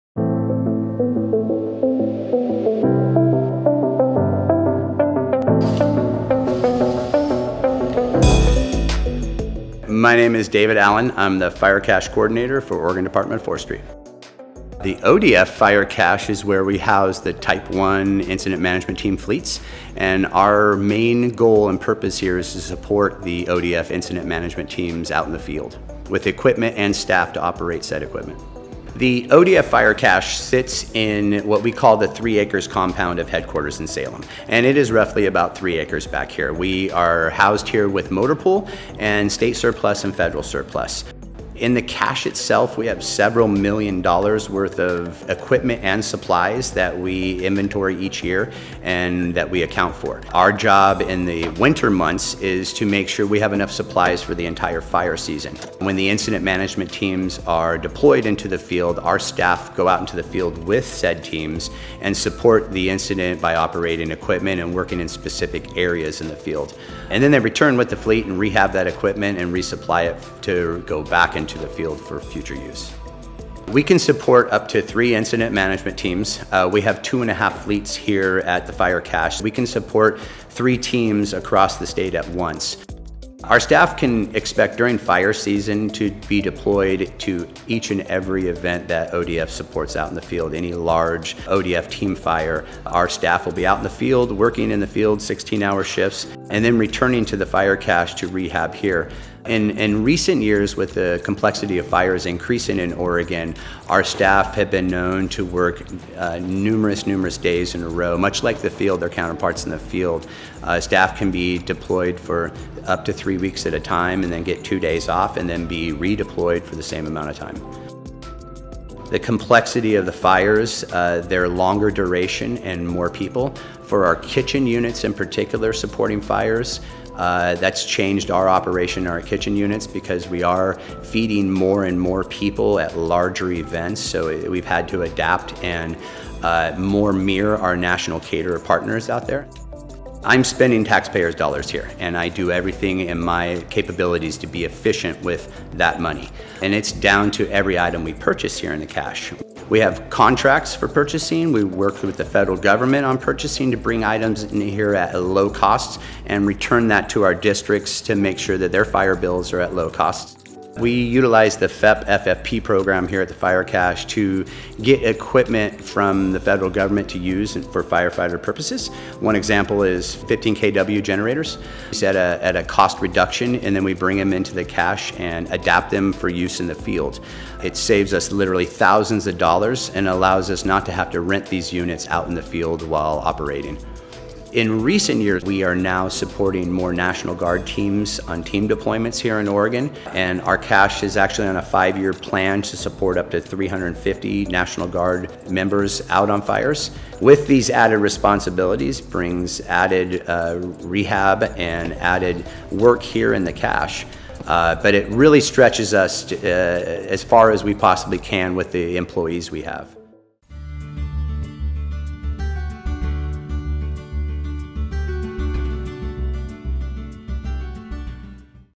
ODF Fire Cache behind-the-scenes tour
With hundreds of firefighters to provide showers, meals, equipment and any other needs for on an incident, the ODF Fire Cache is there. The fire cache stores everything from paperclips to kitchen trailers to sleeping bags, and is ready to send out resources at any time. The team works year round to rehab, restock, organize, and build supplies, and is dedicated to making sure ODF is prepared for anything.

For more information about the Oregon Department of Forestry, please